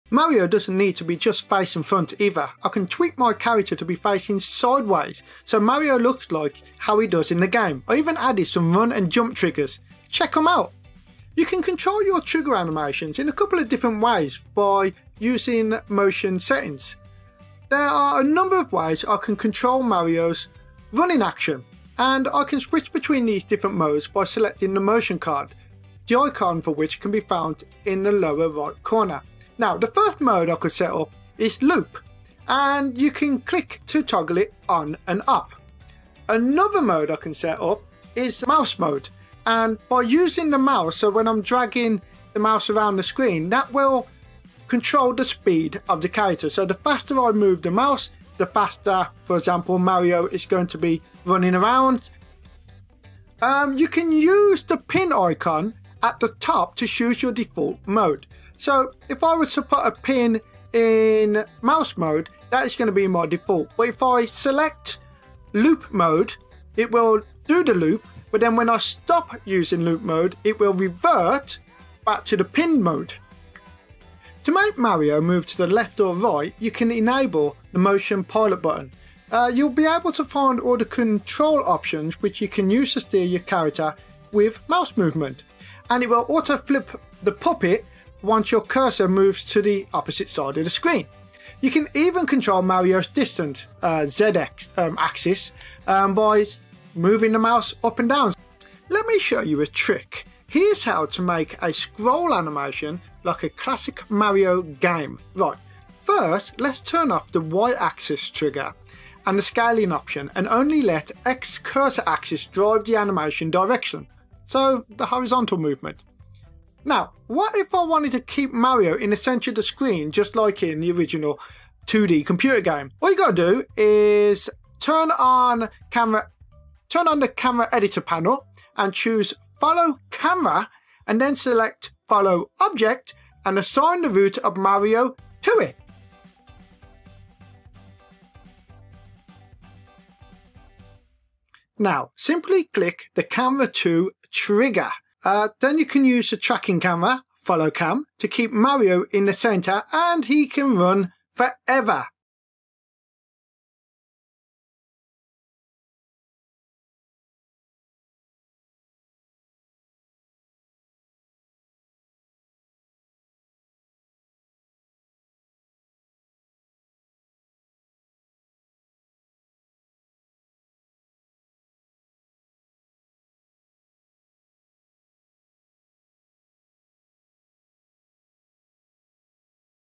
Reallusion Cartoon Animator 5 Motion Pilot
In this preview Scott talks about how you can use Reallusion CTA5 Motion pilot to operate your 2D Digital Puppet